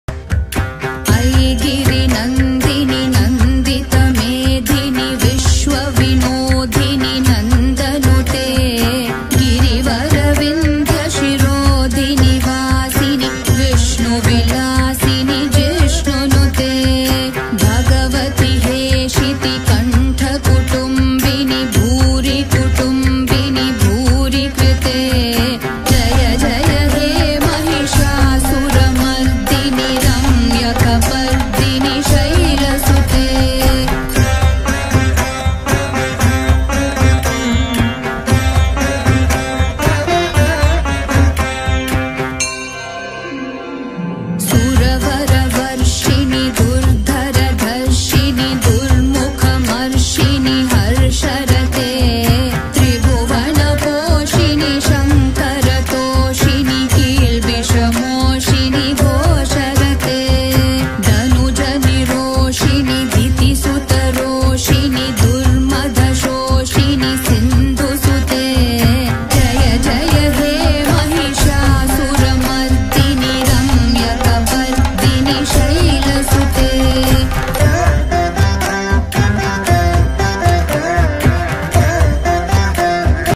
villakku poojai...
pournami special (villakku poojai)  "visinu Maya Devi Kovil" pallikaranai.